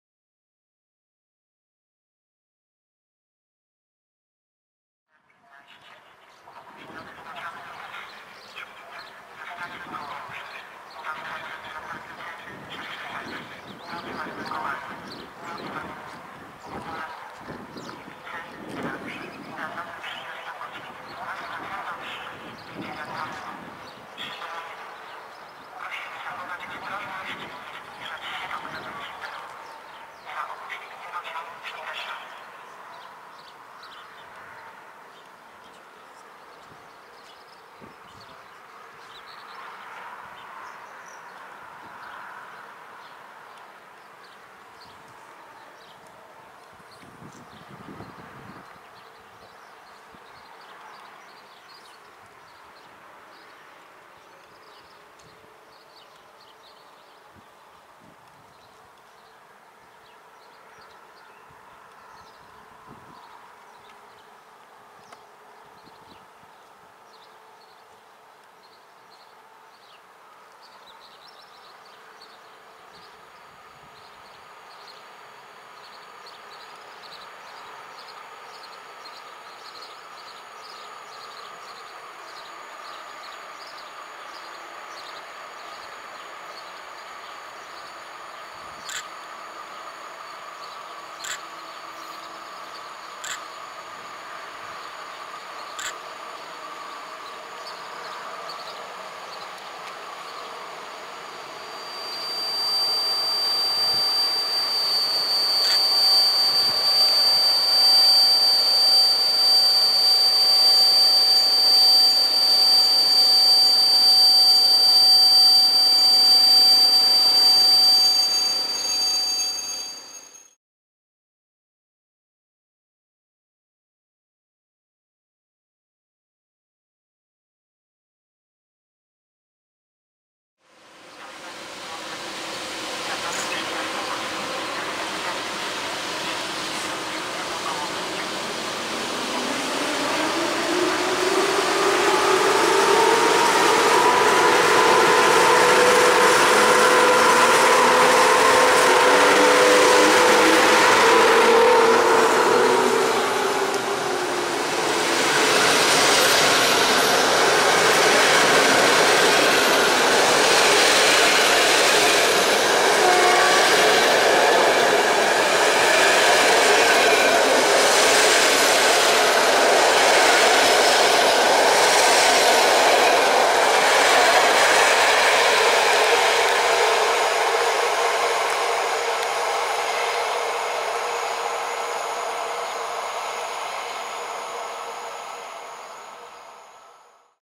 EP07-1017 z opóźnionym ponad 200 minut IC 84108/9 Szkuner do Katowic. +Zapowiedź i RP1
EP07-1017 z opóźnionym ponad 200 minut i prawie pustym składem pociągu IC 84108/9 Szkuner relacji Kołobrzeg/Słupsk - Katowice na stacji Żmigród.

Województwo: Dolnośląskie
Miejscowość: Żmigród
Data: 17 Czerwiec 2019r.
Godzina: 15:35
Przewoźnik: PKP Intercity
D29: 271 (Wrocław - Poznań)
Montaż: Camtasia Studio